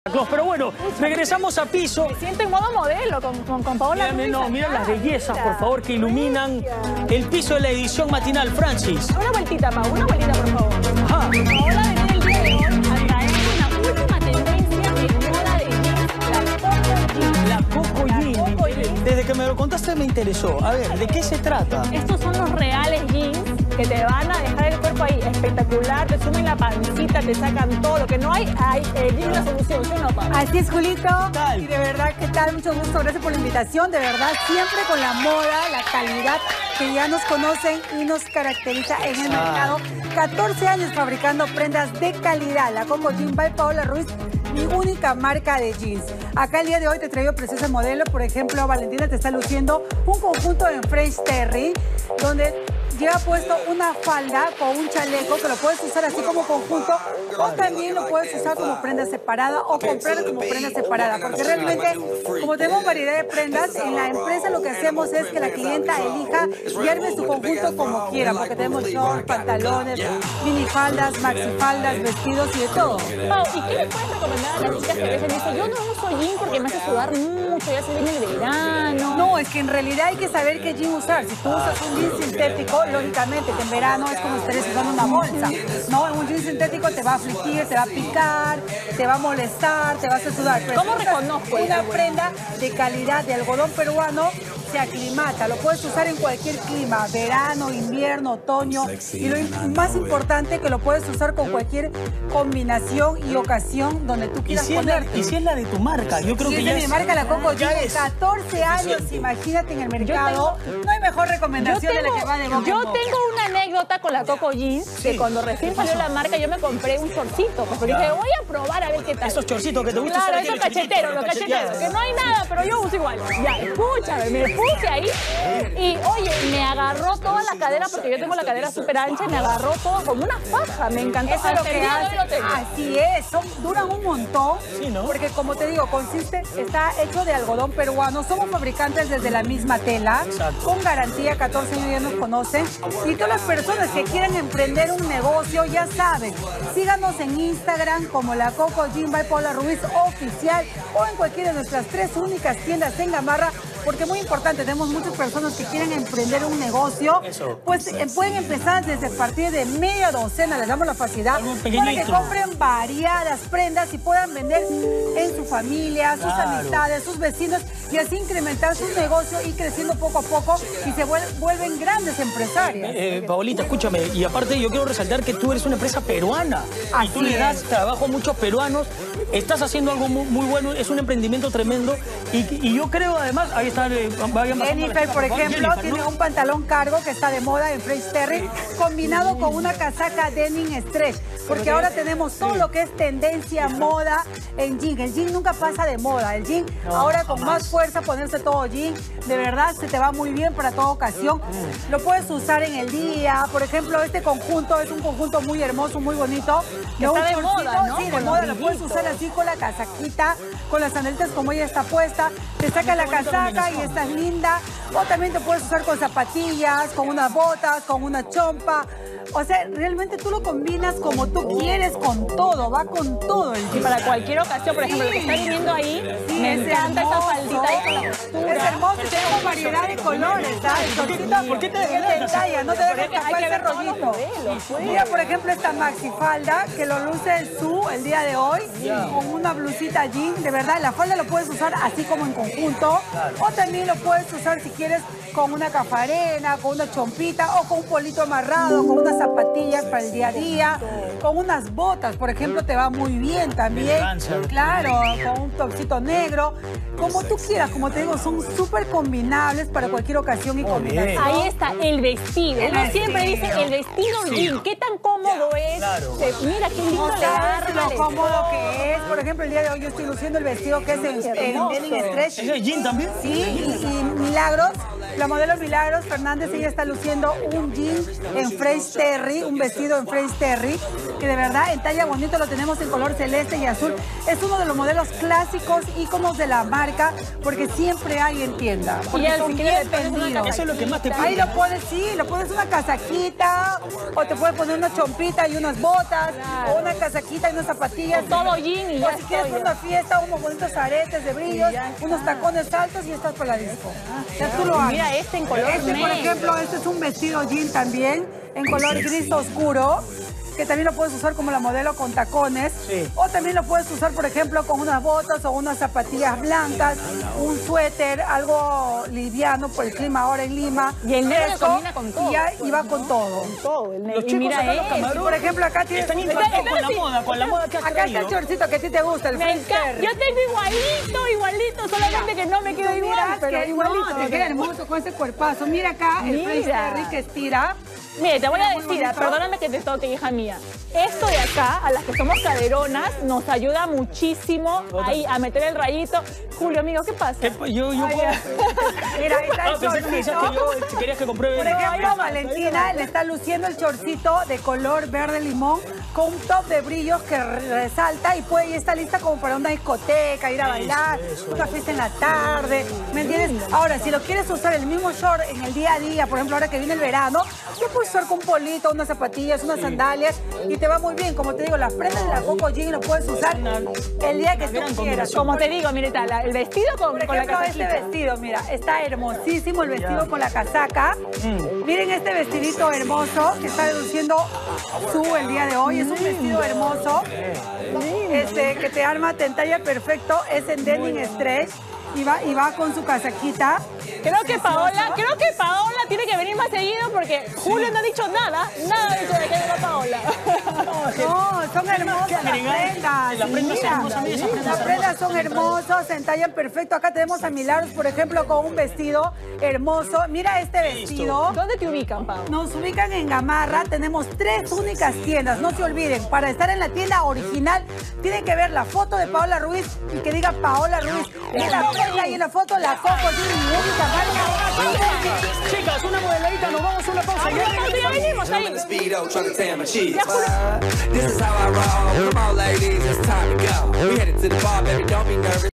Haz Platita en Navidad con los Jeans
Fuente ATV

Coco Jeans de Paola Ruiz
Paola Ruiz nos trae lo ultimo en Jeans